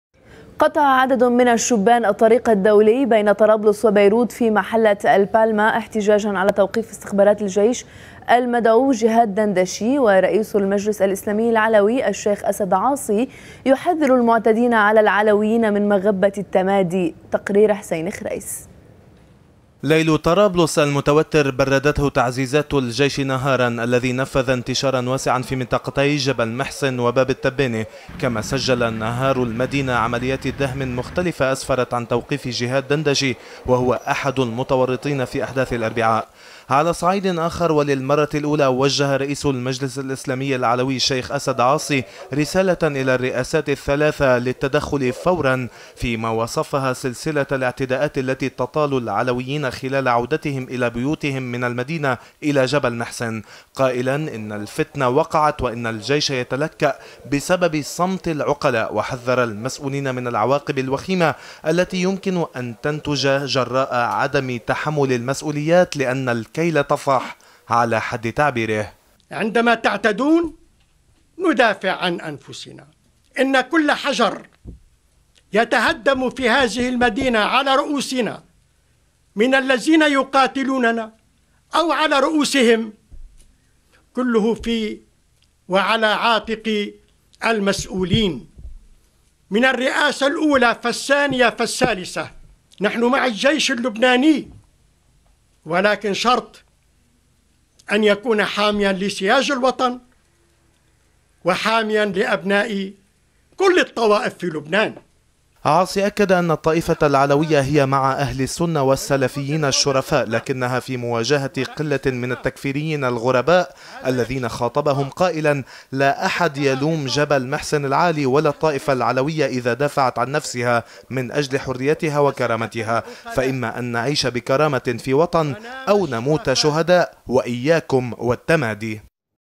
إنتشار كثيف للجيش
قطع عددٌ من الشبان الطريقَ الدوليَ بين طرابلس وبيروت في محلة ال"بالما"، احتجاجا على توقيفِ استخباراتِ الجيش المدعو جهاد دندشي . ورئيسُ المجلسِ الإسلامي العلوي الشيخ أسد عاصي يحذر المعتدين على العلويين من مغبةِ التمادي. تقرير حسين خريس.